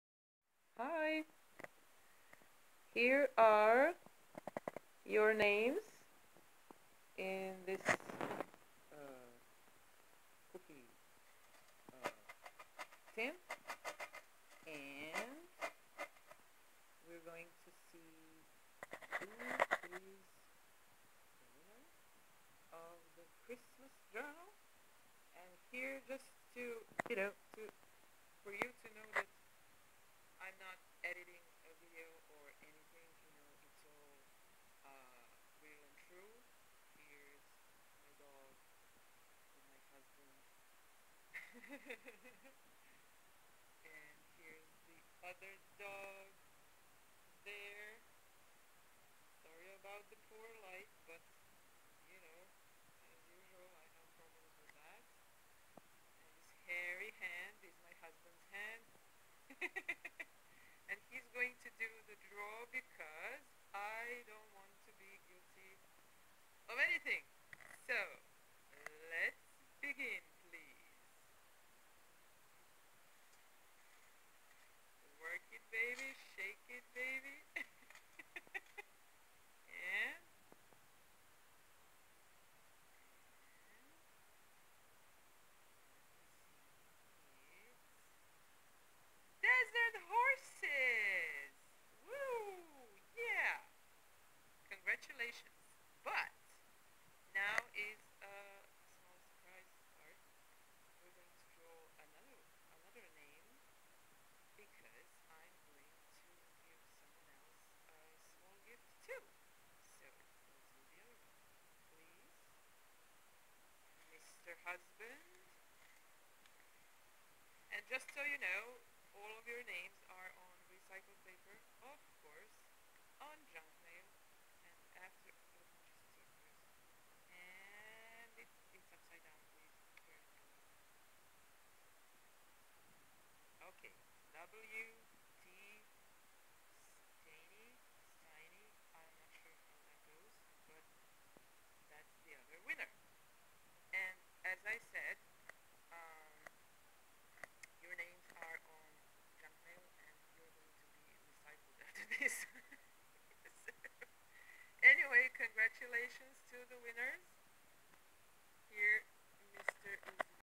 Giveaway Winner (A horrible video, SORRY!!!)
I am SO SORRY about the sound in this video but I didn't want to do another one because that wouldn't be fair, right? But at least you can see who the winner is.